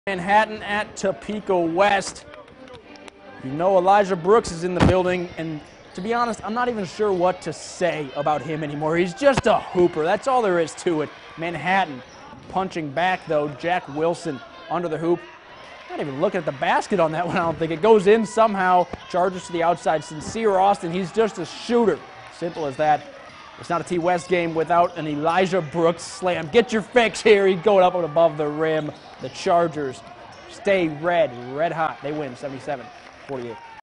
Manhattan at Topeka West- Boys
Highlights from Topeka West's win over Manhattan in boys basketball.